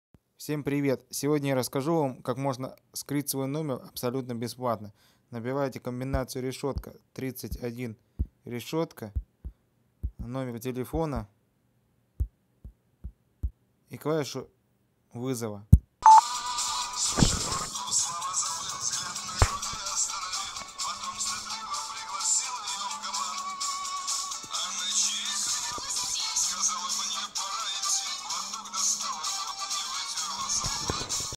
Как скрыть свой номер бесплатно
Лайфхак как скрыть свой номер телефона абсолютно бесплатно работает только на андройде